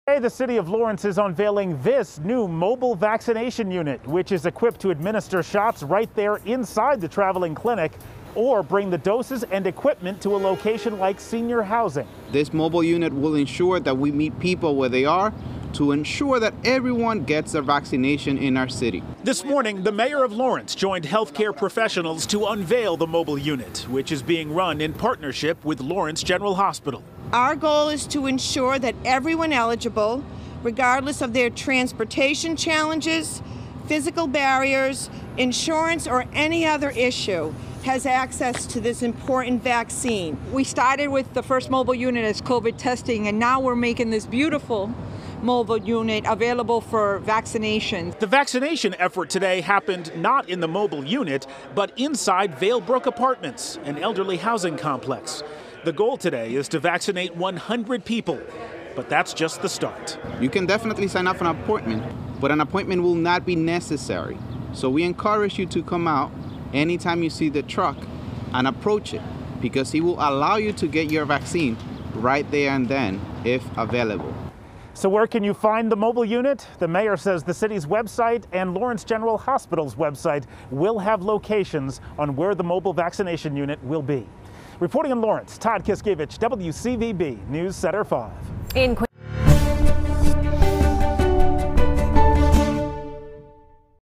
Lawrence rolls out mobile vaccine clinic
Lawrence General Hospital is pushing a new effort to get more people vaccinated:  a a mobile vaccine clinic to bring the shots directly to residents.